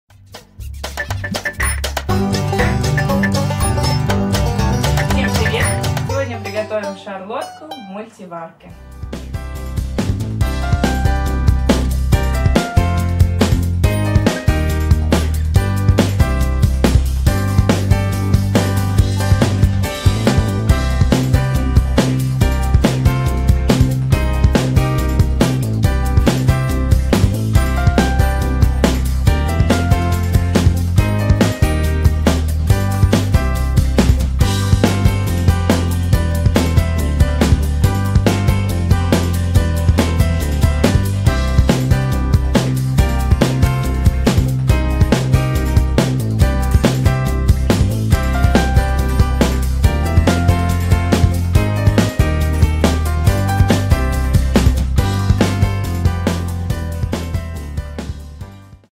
Шарлотка в Мультиварке с Яблоками | Быстрый и Очень Вкусный Рецепт
Ингредиенты:
2 средних яблока
2 яйца

Приготовление:
1.Разбиваем куриные яйца в глубокое блюдо, насыпаем сахар, хорошо взбиваем. Яйца лучше заранее достать из холодильника, чтобы они согрелись, так они легче взобьются с сахаром.
2.Для того, чтобы тесто получилось легкое и пышное, просеиваем муку несколько раз. Вводим в получившуюся массу, перемешивая венчиком. Добавляем соду, гашеную уксусом в чайной ложке.
3. Яблоки моем, обсушиваем и нарезаем. Лучше всего использовать для этого пирога антоновку. Если кожура у яблок жесткая, то её необходимо очистить. Нарезаем на дольки или кубики, как больше нравится.
4. Чашу смазываем растительным маслом, выкладываем нарезанные яблоки и заливаем их тестом.
5. Включаем мультиварку на режим выпечка и готовим 25 минут. Даем пирогу немного остыть, переворачиваем, посыпаем сахарной пудрой.
Приятного аппетита!